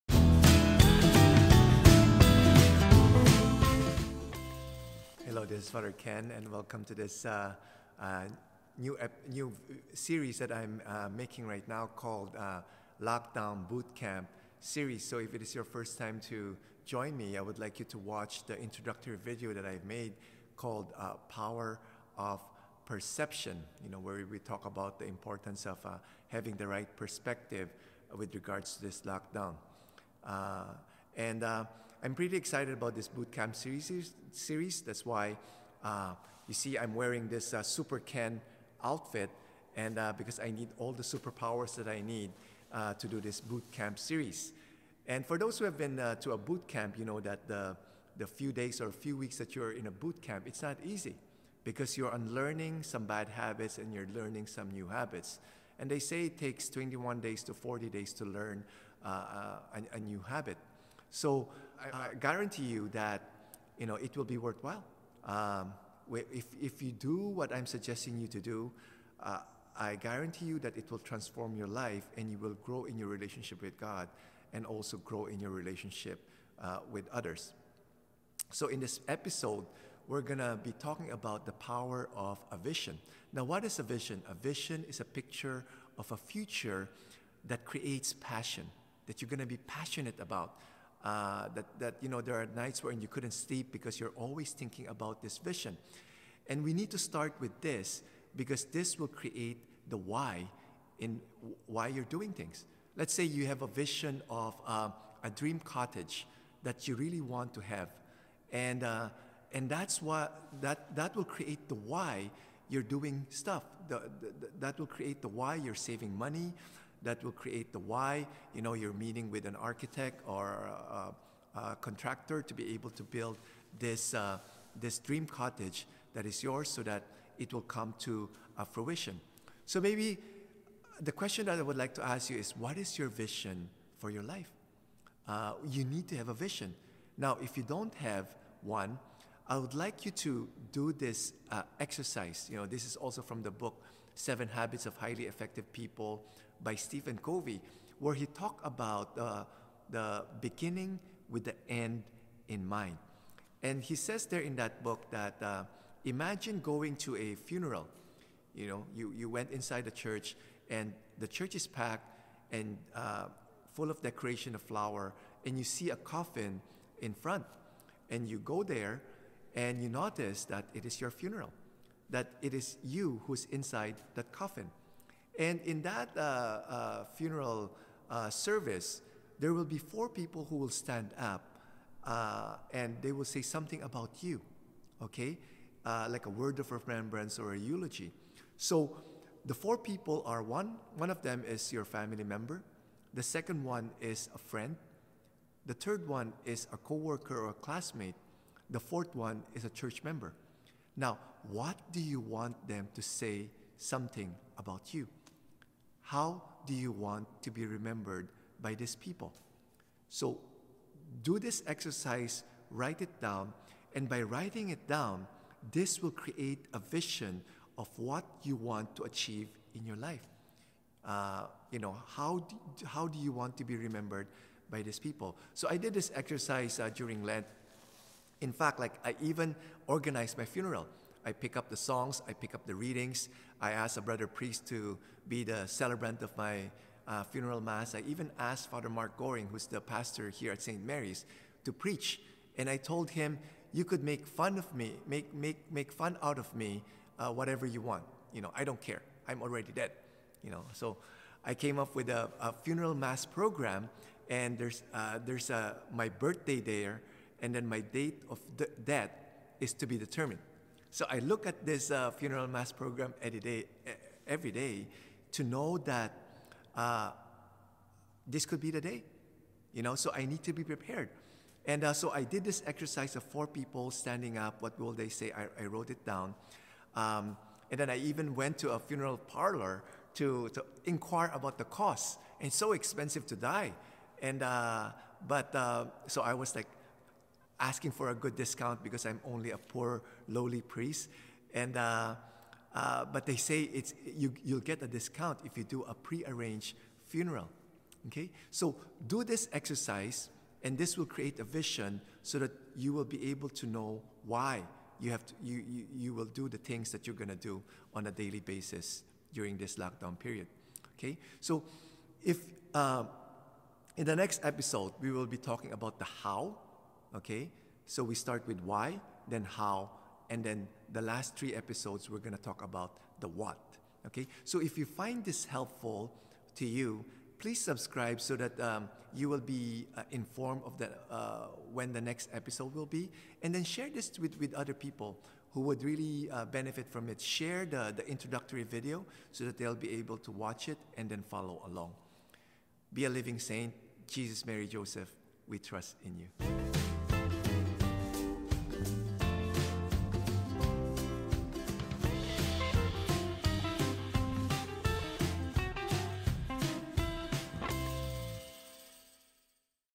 EP 1 - Power Of Vision
"Lockdown Bootcamp Series"

Do you want to make the most of the time this lockdown period?

Fr. Kenneth Lao, CC is a priest of the Companions of the Cross